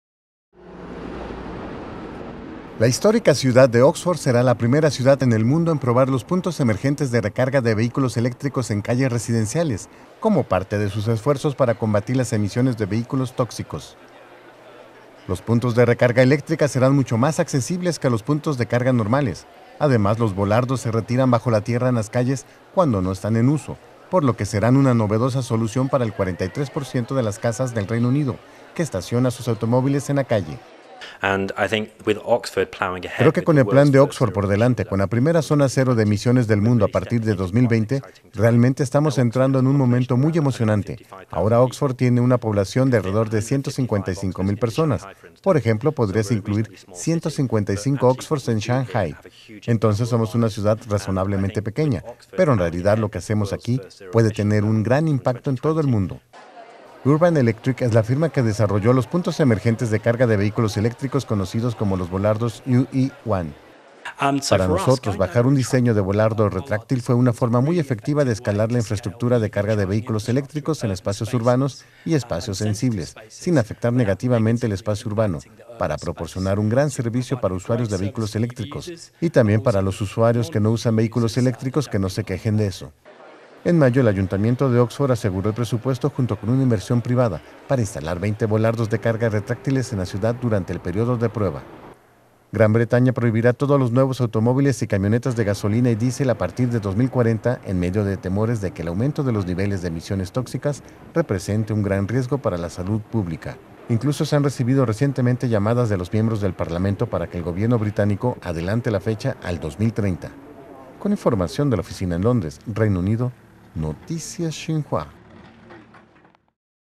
Oxford prueba puntos de carga de vehículos eléctricos
La histórica ciudad de Oxford será la primera en el mundo en probar los puntos emergentes de recarga de vehículos eléctricos en calles residenciales como parte de sus esfuerzos para combatir las emisiones de vehículos tóxicos.